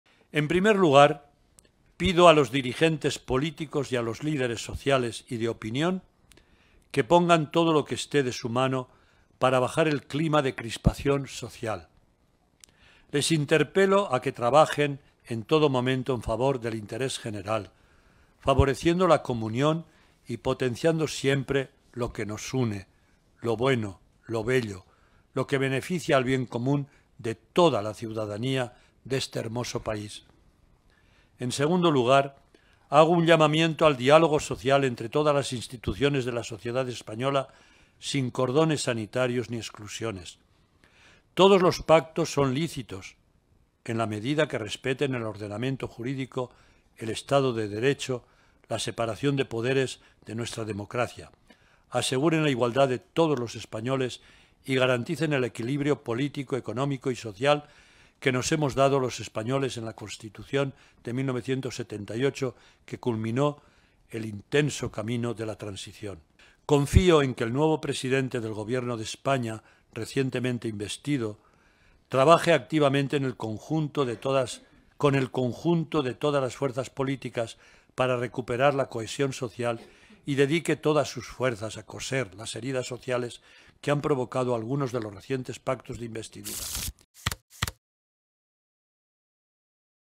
El cardenal Omella pide a Sánchez dedicar "sus fuerzas a coser las heridas sociales de sus pactos"
Declaraciones presidente y el secretario general de la Conferencia Episcopal, el cardenal Juan José Omella, quien ha afirmado este lunes que confía en que el nuevo presidente del Gobierno de España, Pedro Sánchez, recientemente investido, "trabaje activamente con el conjunto de todas las fuerzas políticas para recuperar la cohesión social y dedique todas sus fuerzas a coser las heridas sociales que han provocado algunos de los recientes pactos de investidura".